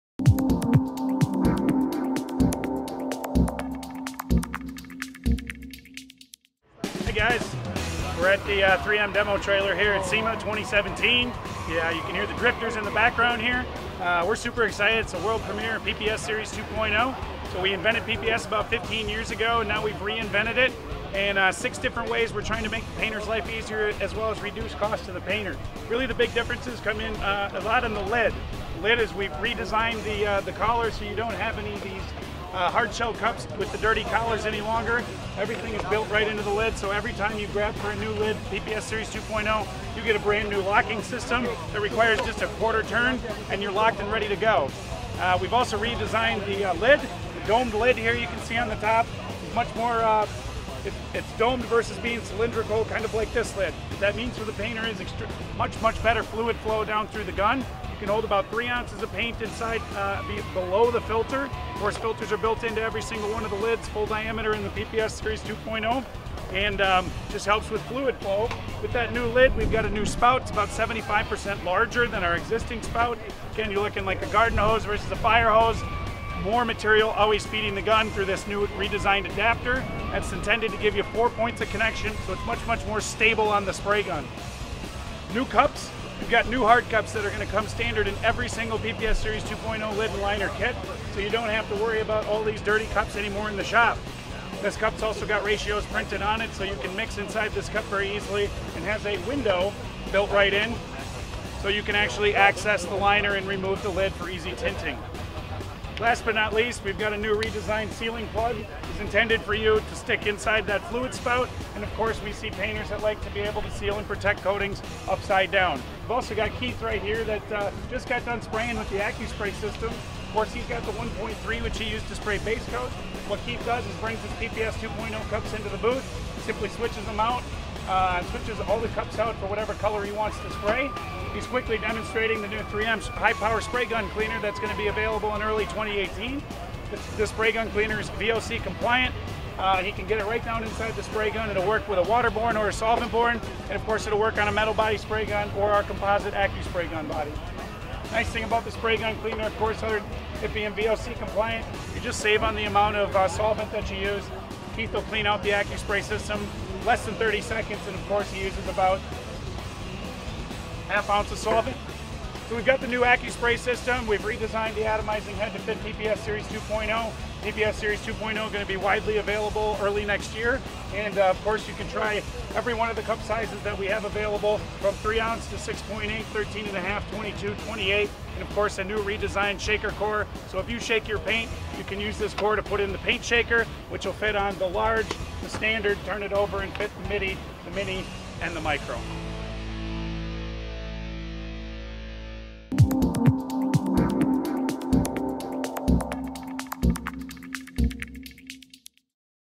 3M™ PPS™ 2.0 Series Spray Cup System at SEMA 2017
Andrew Boyd of 3M Collision Repair Solutions talks through the six improvements for the 3M™ PPS™ 2.0 Series Spray Cup System at the 2017 SEMA Show 3M Demo Truck!